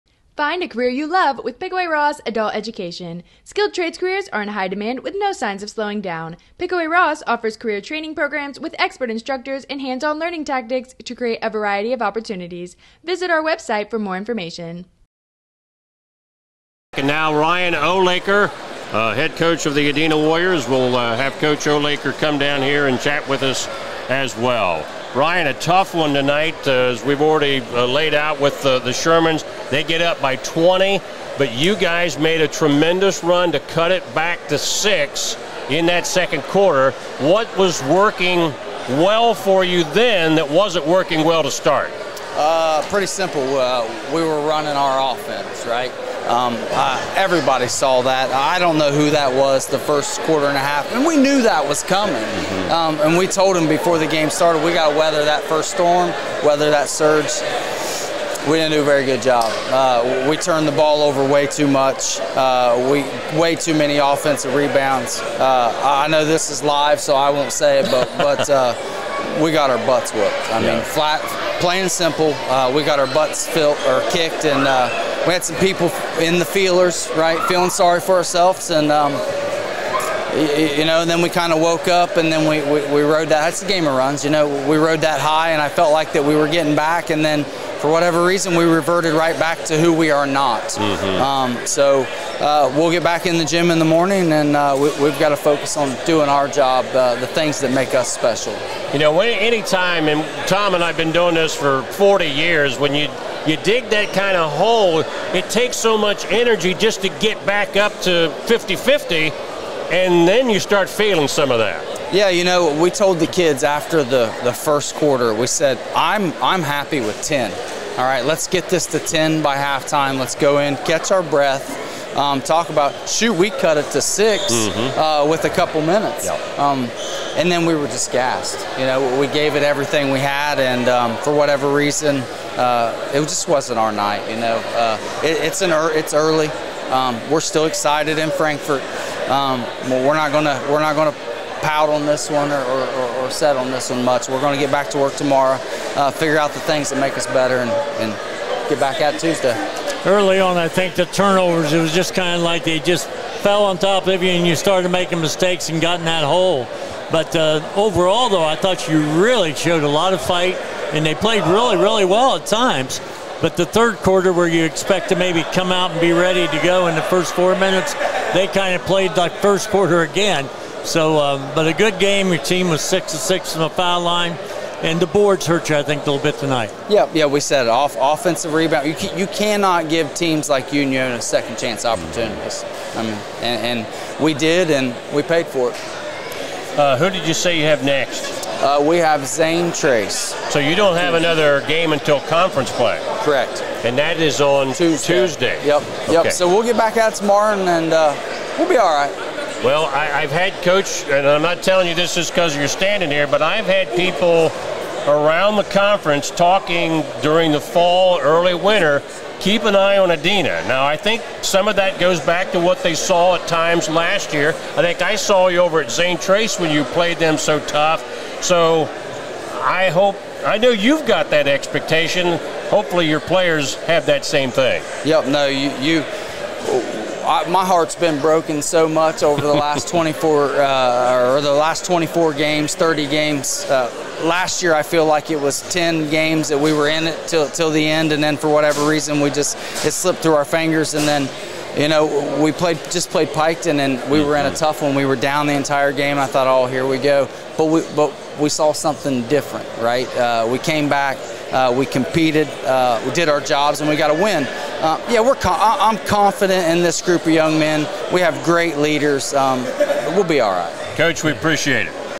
Litter Media Extras: Adena's Ryan Olaker Talks About the Warriors' loss at Unioto
The Unioto Shermans struck early in the contest and earned a 61-43 victory over Adena. The Warriors' head coach, Ryan Olaker talks about the game on Litter Media RADIO's post game show.